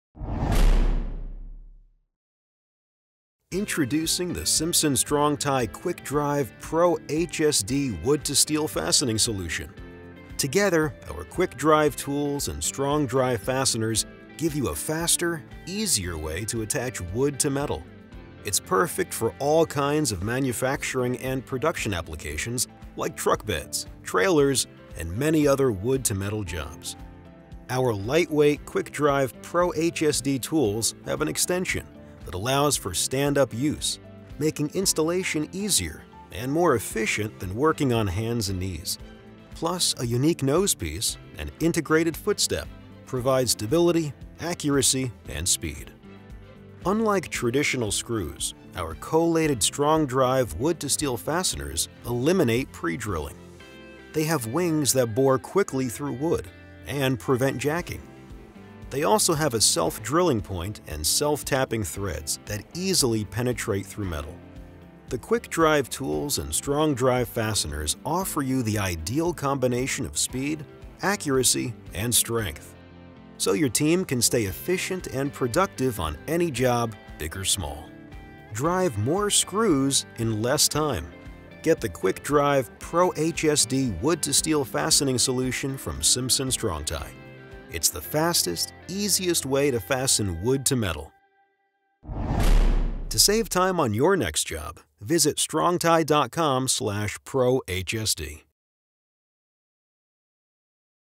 Install Truck and Trailer Beds Faster With This Wood-to-Steel Fastening Solution
Install truck and trailer beds faster with the Simpson Strong-Tie® Quik Drive® PROHSD Wood-to-Steel fastening solution.

Here’s a smart, reliable and easy way to install truck and trailer beds. Our Quik Drive PROHSD and PROHSDM tools have an optional extension and footstep for stand-up use, eliminating the strain of working on hands and knees. The PROHSDM also features a unique nosepiece for greater stability and accuracy. Save time by using the Quik Drive tool with collated strips of Strong-Drive® TB Wood-to-Steel screws for steel up to ¼" thick, or with Strong-Drive TF Wood-to-Steel screws for thicker steel up to 15/32". Like other solutions from Simpson Strong-Tie, the Quik Drive PROHSD Wood-to-Steel fastening solution is widely available and backed by our expert service and technical support.